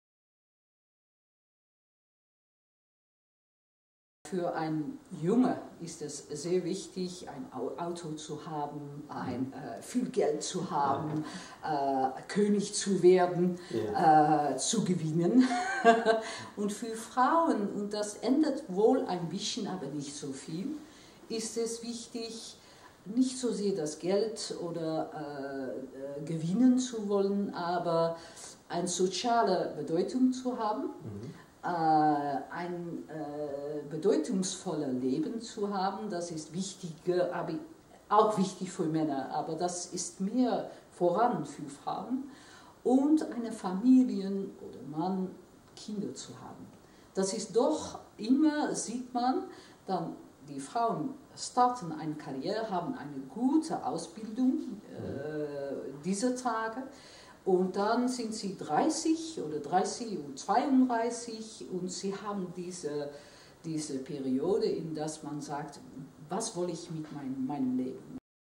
Creating Corporate Cultures - Ina Brower zu Karrieremotiven von Männern und Frauen
Ina Brouwer, frühere Präsidentin der niederländischen Taskforce Childcare / Education, erörterte mit dem Kompetenzzentrum Führung und Unternehmenskultur der Bertelsmann Stiftung Fragen zu Unternehmenskultur. Sie ging auf die Besonderheiten weiblicher Karrieren ein und machte Vorschläge, was Organisationen tun können, um Frauen bei ihrer Arbeit zu unterstützen. Im Film spricht Ina Brouwer über die unterschiedliche Karrieremotive von Männern und Frauen.